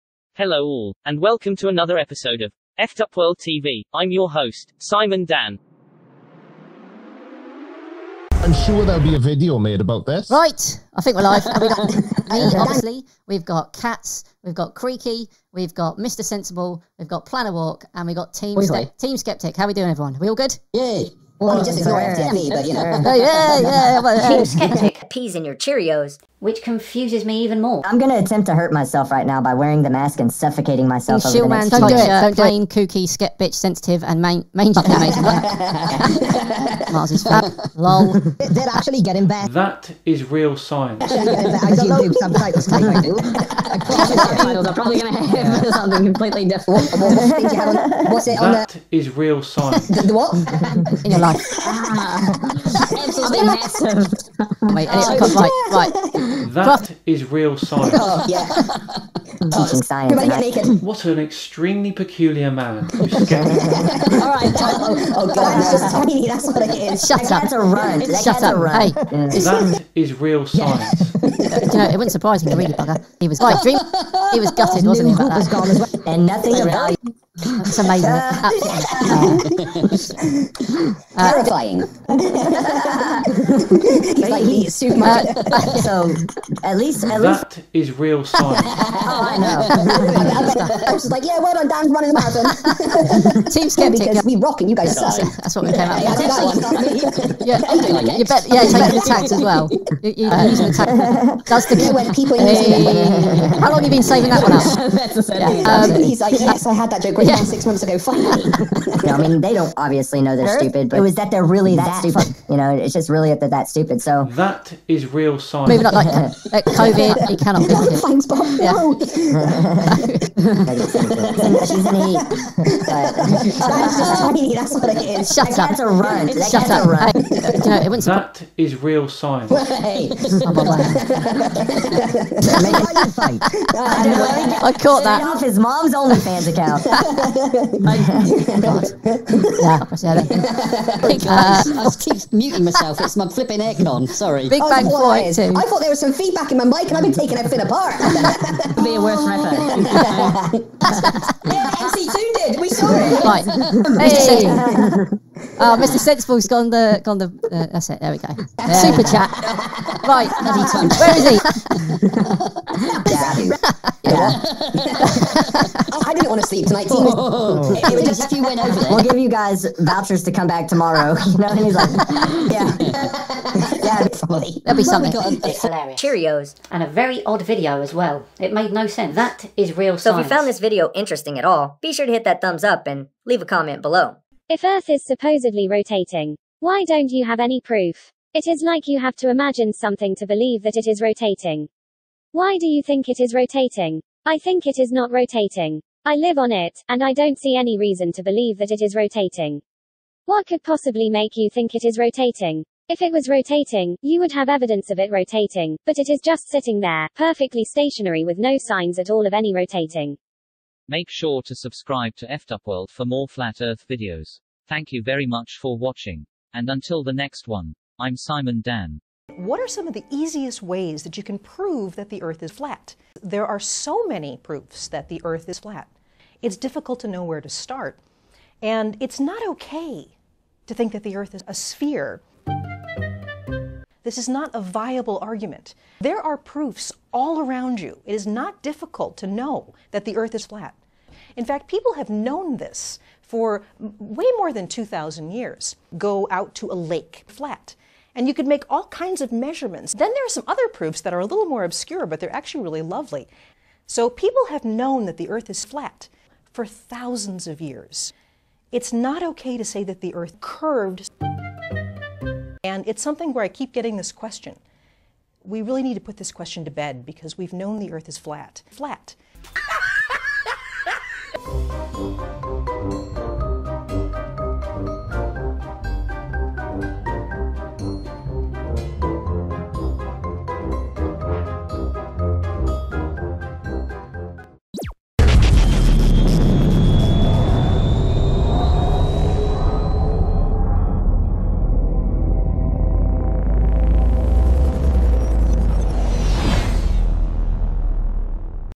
The Giggling ScimanDan Debunker Clan(and more)
These guys think the earth is a sphere.
They also think they debunked Flat Earth.
Keep giggling boys.  You won't be laughing very long.
Earth is observably flat and stationary.